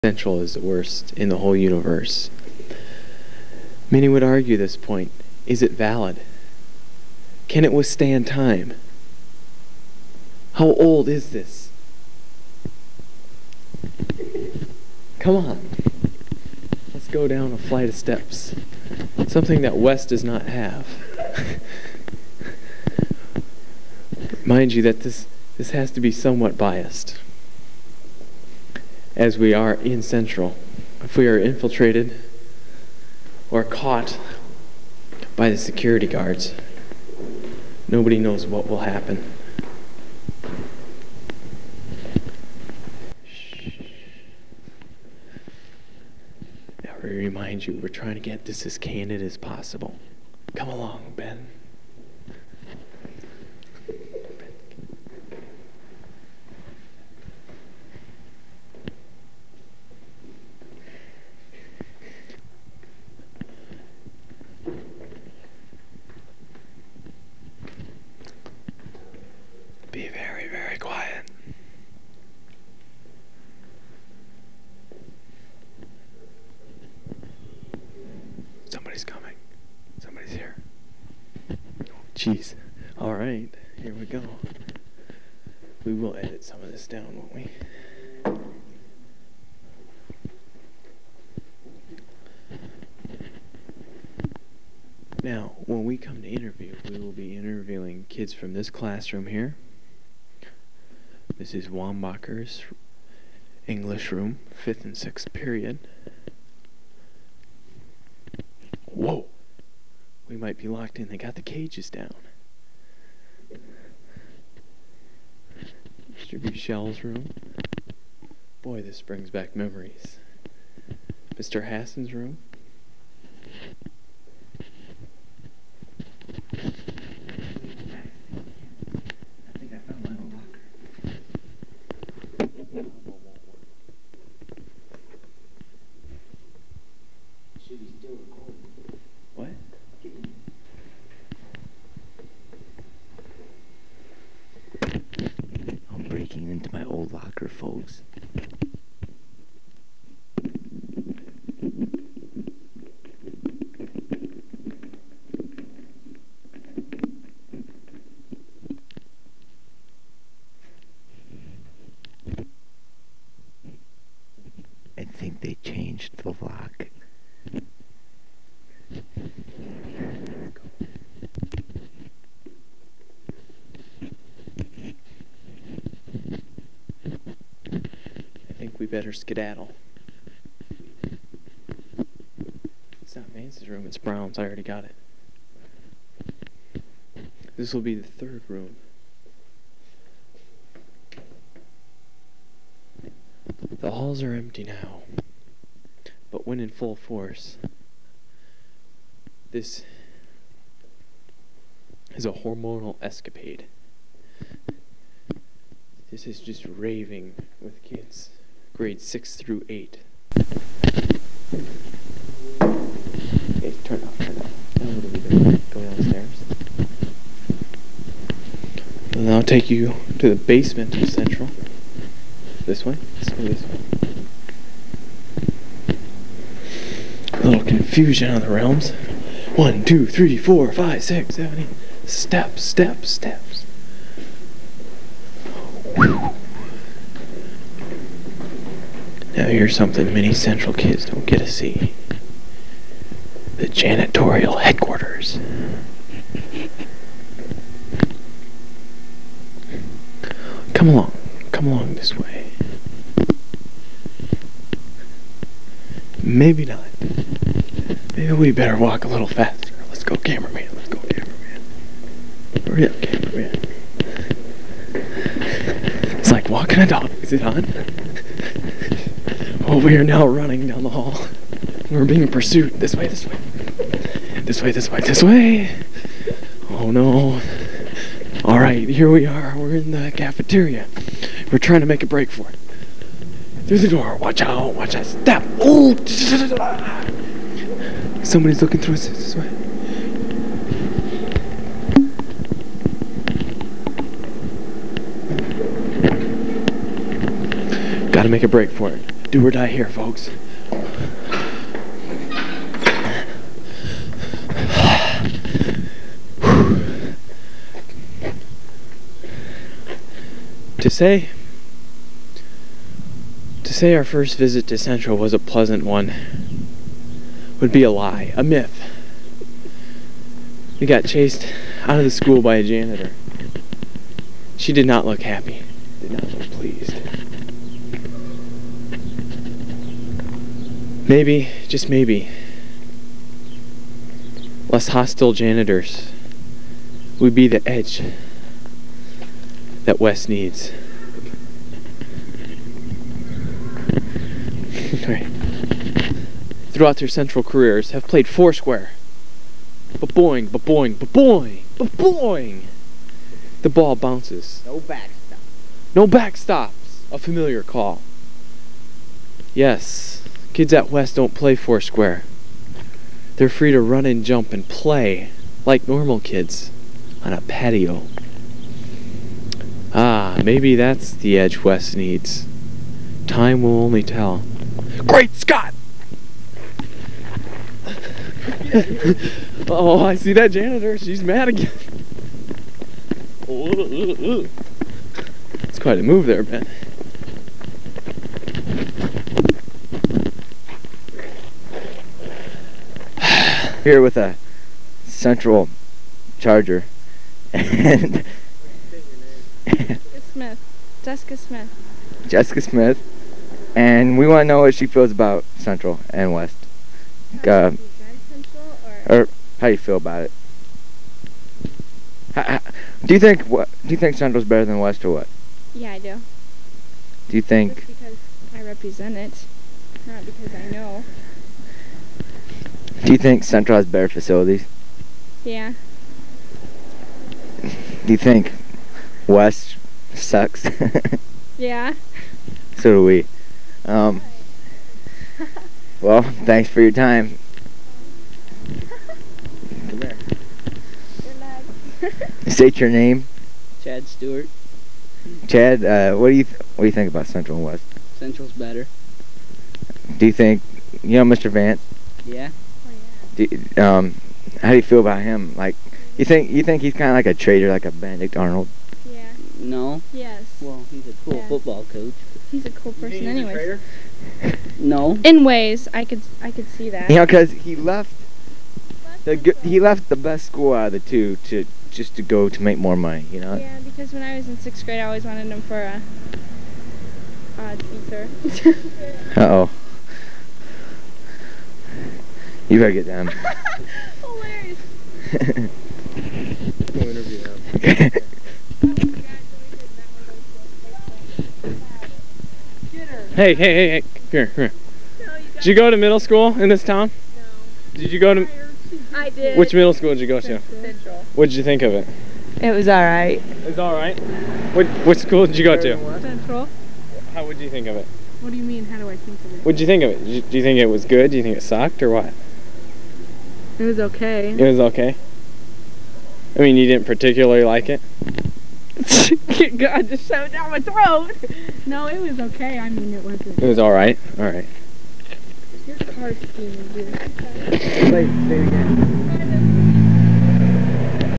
CENTRAL VS. WEST PART 1 - WALKING CENTRAL
Max & Ben walk the halls of Central after hours and pester some teens on the street.